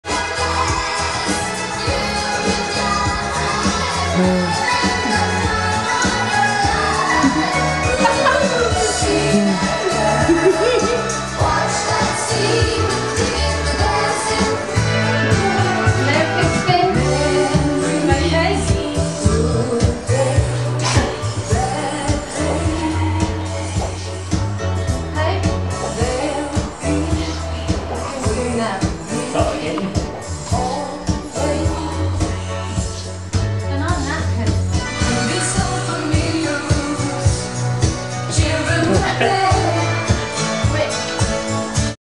Lyn & Hazel Dancing at New Years Eve - 2007
Abba on the stereo, plenty of fizzy being drunk... what better way is there than ballroom dancing to see in the new year!